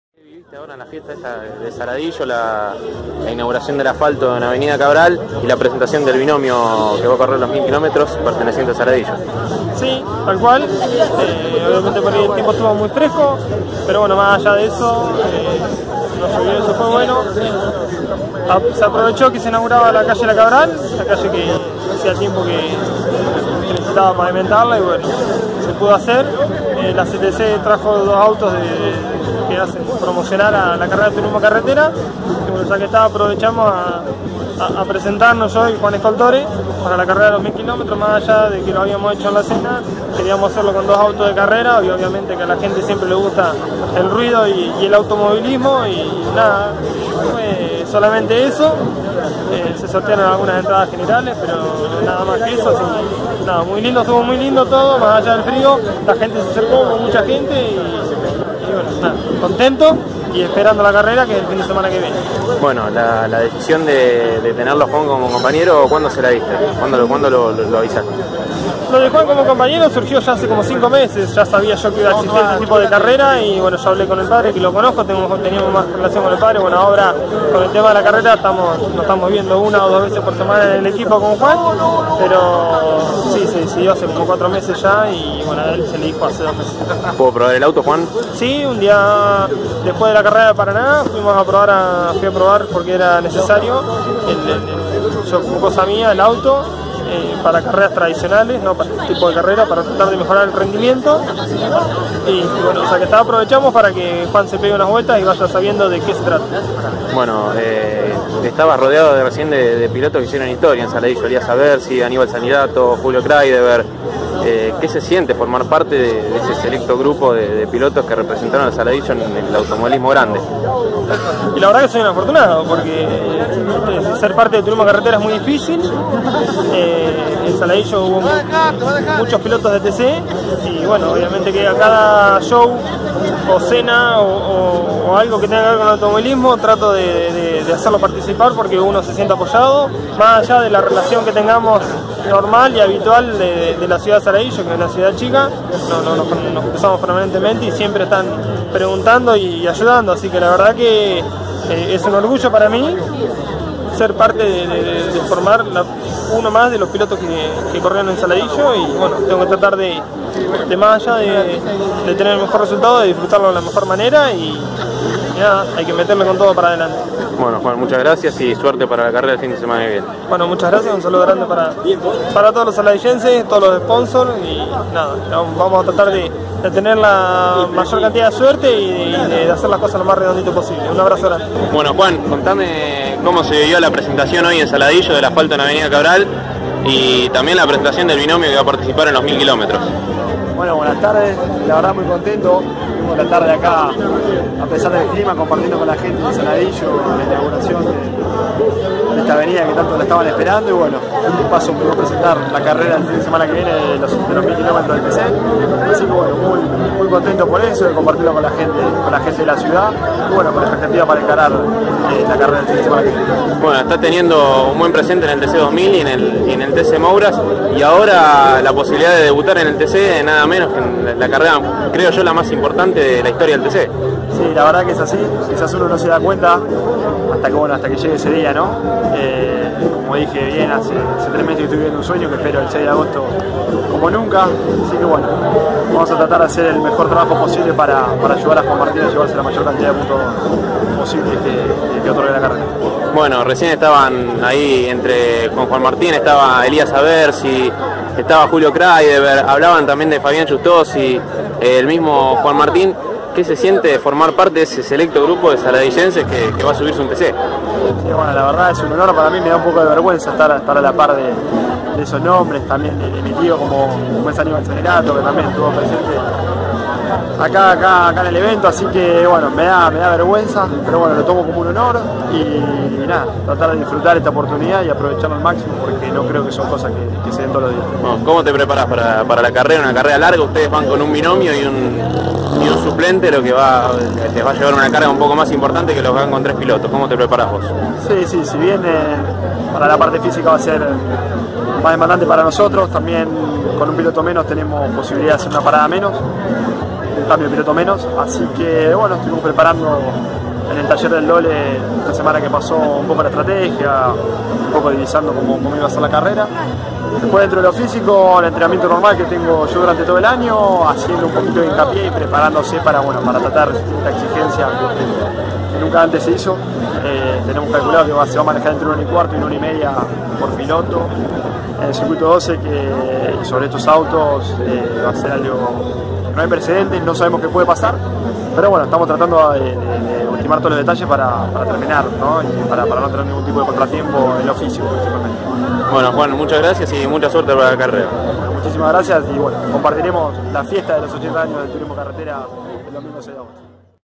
Juan Martin Bruno y Juan Scoltore se presentaron en Saladillo
Juan Martín Bruno y Juan Scoltore se presentaron en Saladillo antes de los 1000km del Turismo Carretera en Buenos Aires. En tanto, los pilotos locales participaron de la inauguración del reasfaltado de parte de la Avenida Cabral.